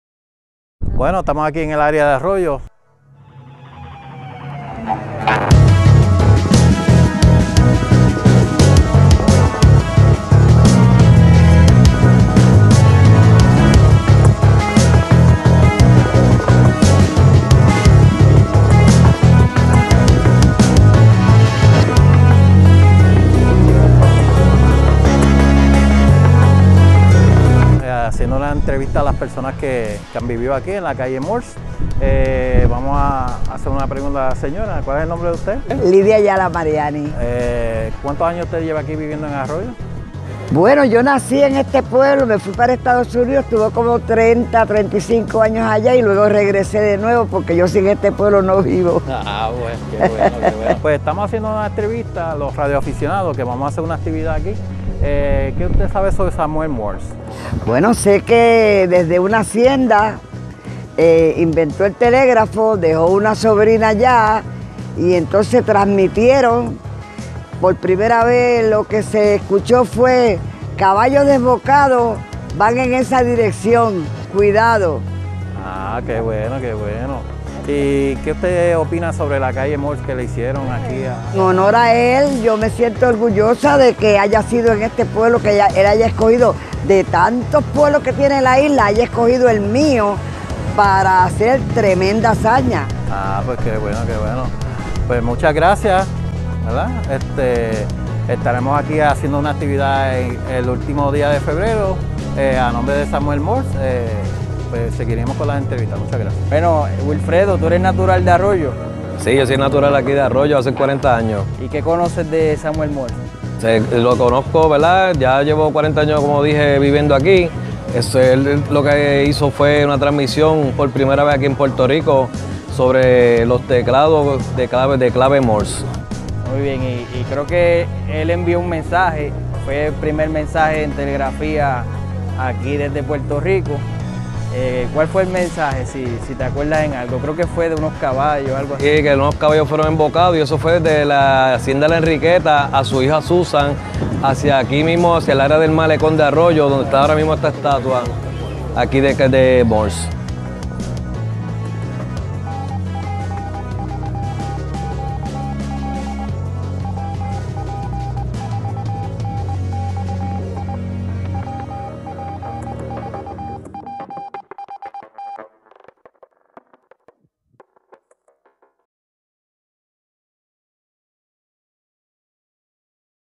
Inventor del código Morse CW Samuel Morse en Arroyo Puerto Rico
Inventor del código Morse CW. Aniversario 159 de la Comunicación por telegrafía en Puerto Rico por el SR. Samuel FB Morse el 1 de marzo de 1859.